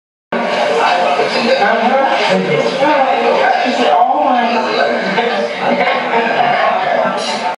(cover) by Voodoo
Arline Rowe's birthday party on 06/14/2014 in Vero Beach featuring the musical interpretations of classic songs by Voodoo featuring Ed Lewis on drums, Bruce Feller on Bass, Paul Modesto on Lead guitar and vocals and Lee R. Rathbun on Guitar and vocals.